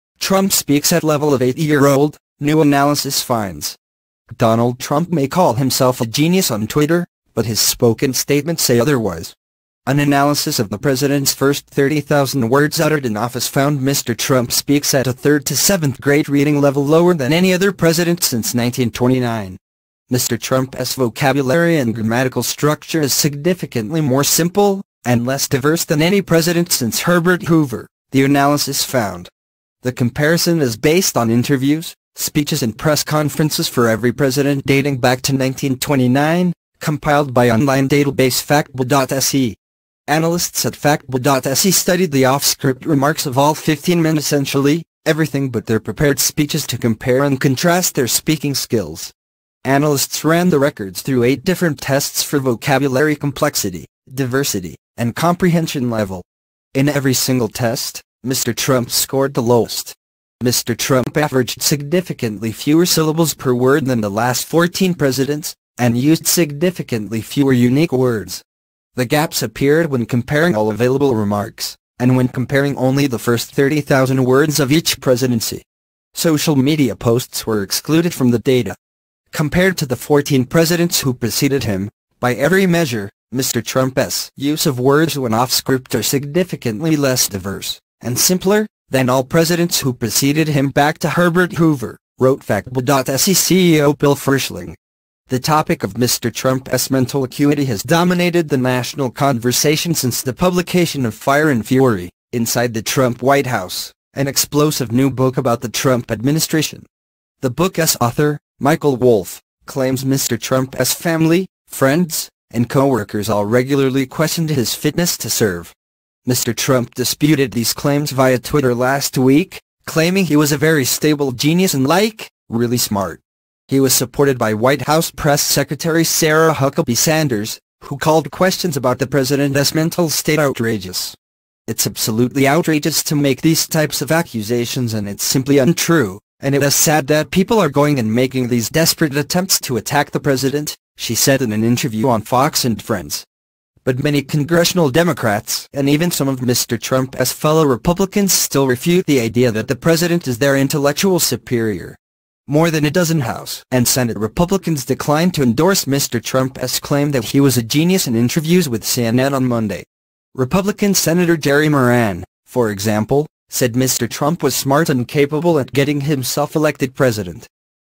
Trump speaks at level of 8 year old, new analysis finds
Donald Trump may call himself a genius on Twitter, but his spoken statements say otherwise.

An analysis of the President's first 30,000 words uttered in office found Mr Trump speaks at a third- to seventh-grade reading level – lower than any other President since 1929. Mr Trump’s vocabulary and grammatical structure is “significantly more simple, and less diverse” than any President since Herbert Hoover, the analysis found.

The comparison is based on interviews, speeches and press conferences for every president dating back to 1929, compiled by online database Factba.se. Analysts at Factba.se studied the “off-script” remarks of all 15 men – essentially, everything but their prepared speeches – to compare and contrast their speaking skills.
Analysts ran the records through eight different tests for vocabulary complexity, diversity, and comprehension level. In every single test, Mr Trump scored the lowest.

Mr Trump averaged significantly fewer syllables per word than the last 14 Presidents, and used significantly fewer unique words. The gaps appeared when comparing all available remarks, and when comparing only the first 30,000 words of each presidency. Social media posts were excluded from the data.

“Compared to the 14 presidents who preceded him, by every measure, [Mr Trump’s] use of words when off script are significantly less diverse, and simpler, than all presidents who preceded him back to Herbert Hoover,” wrote Factba.se CEO Bill Frischling.
The topic of Mr Trump’s mental acuity has dominated the national conversation since the publication of Fire and Fury: Inside the Trump White House, an explosive new book about the Trump administration. The book’s author, Michael Wolff, claims Mr Trump’s family, friends, and coworkers all regularly questioned his fitness to serve.

Mr Trump disputed these claims via Twitter last week, claiming he was a “very stable genius” and “like, really smart”. He was supported by White House Press Secretary Sarah Huckabee Sanders, who called questions about the President’s mental state “outrageous”.
“It's absolutely outrageous to make these types of accusations and it's simply untrue, and it’s sad that people are going and making these desperate attempts to attack the President,” she said in an interview on Fox & Friends.

But many Congressional Democrats – and even some of Mr Trump’s fellow Republicans – still refute the idea that the President is their intellectual superior. More than a dozen House and Senate Republicans declined to endorse Mr Trump’s claim that he was a “genius” in interviews with CNN on Monday.

Republican Senator Jerry Moran, for example, said Mr Trump was “smart and capable at getting himself elected president”. But when asked whether he was a genius, Mr Moran replied: “Got nothing.”